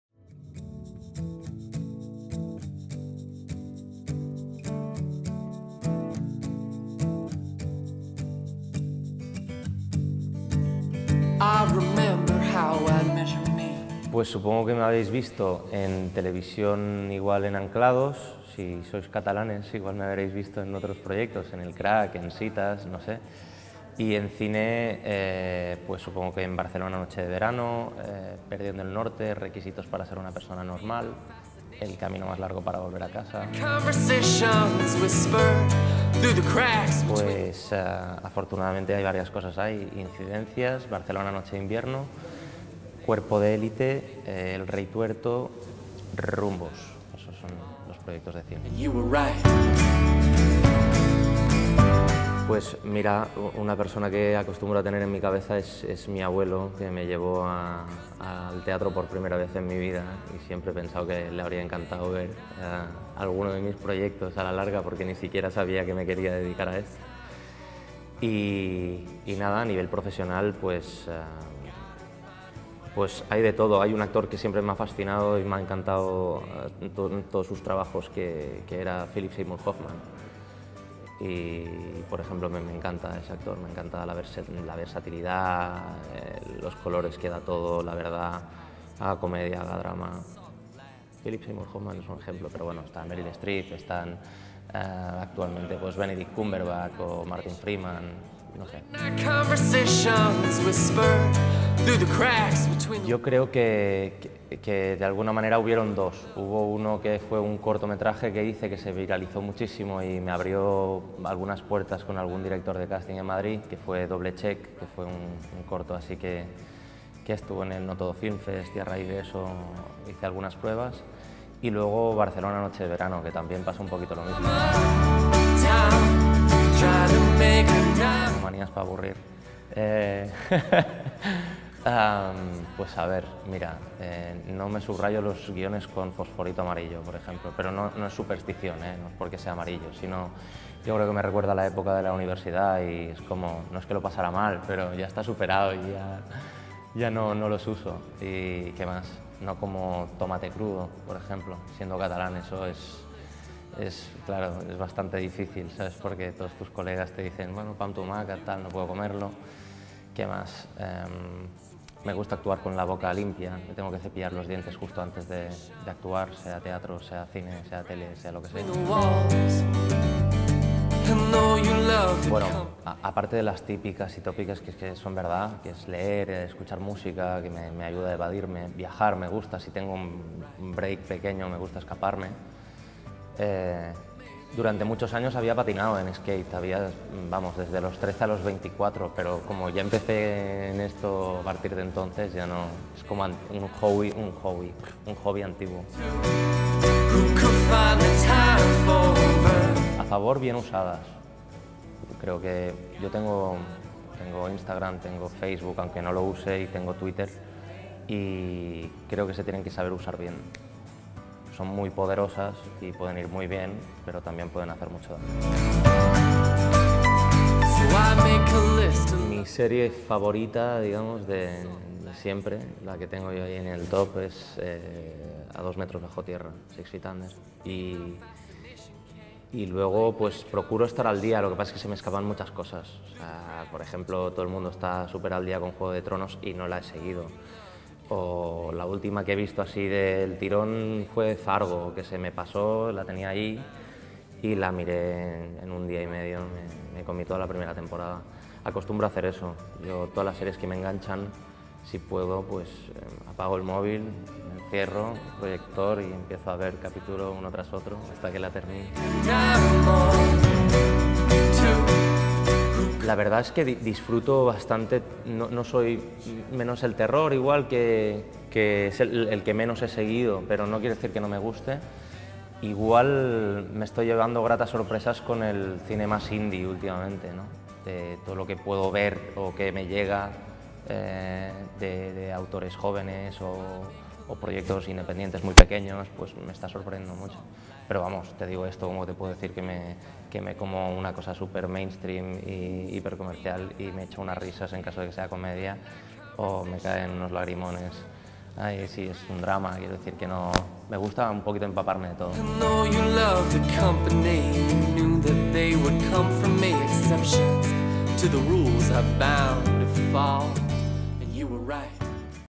Relevo 2015: Miki Esparbe | Fotogramas
“Si un proyecto se presenta como un reto, es que interesa.” Entrevista a Miki Esparbé para el Relevo 2015 de Fotogramas.